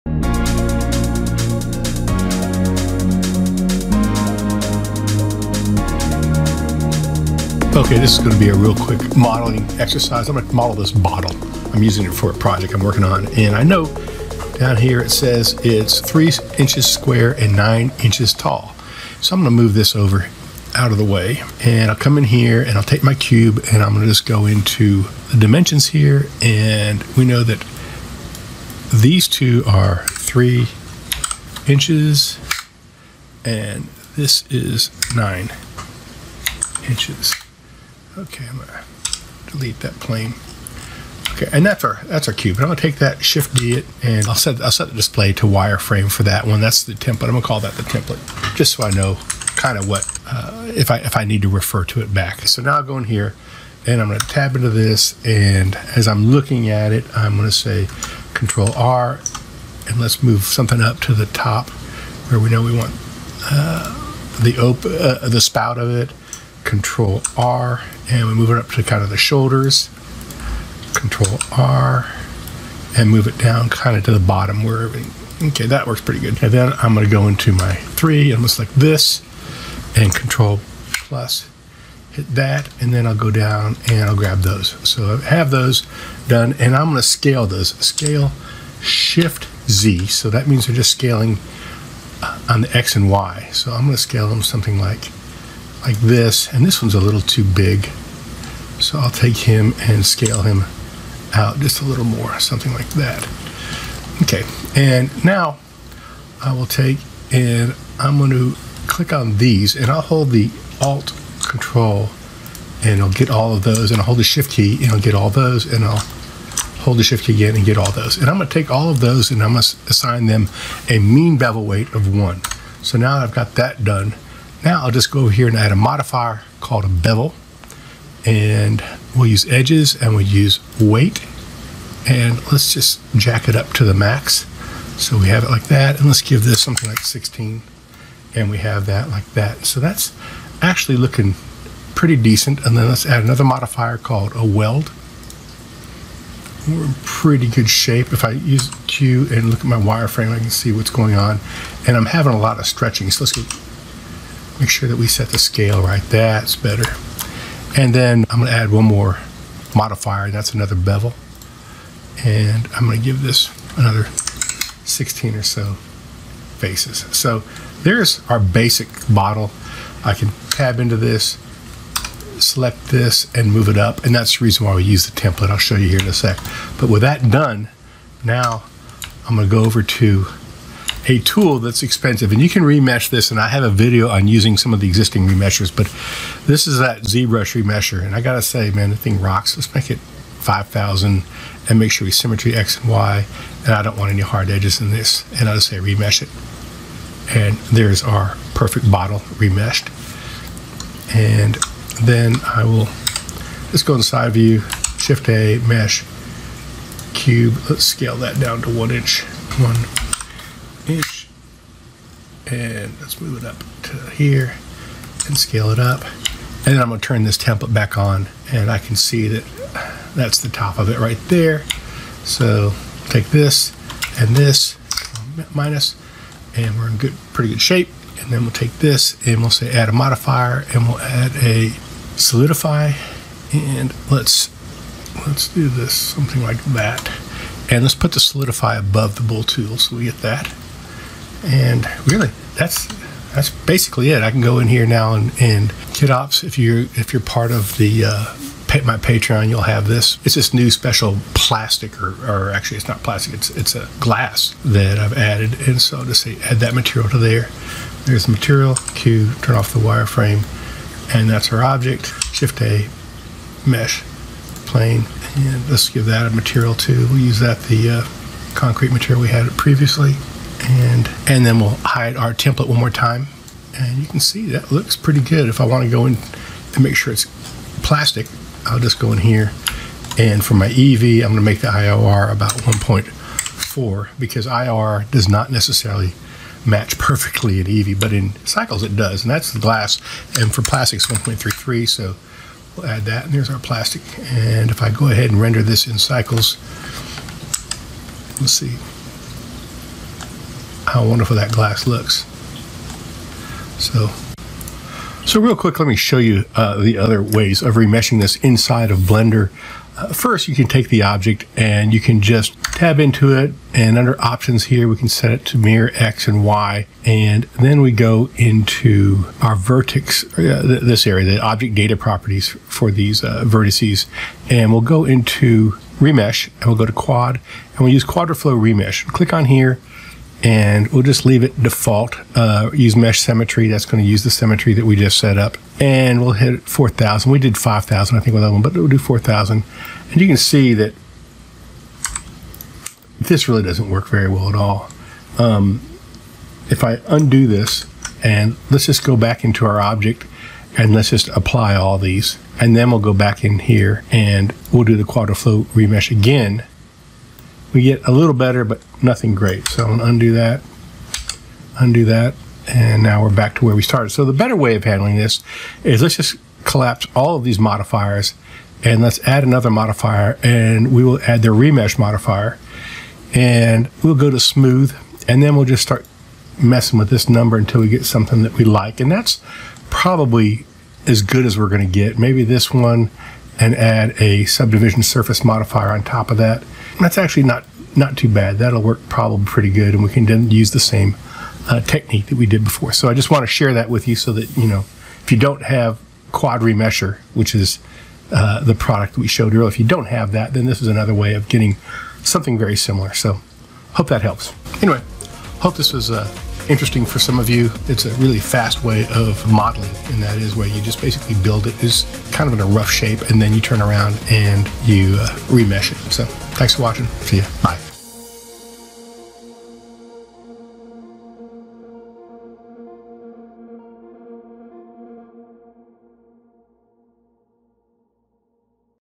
3 Minute Bottle in Blender
Use modifiers and remeshing techniques to create a really fast bottle in Blender!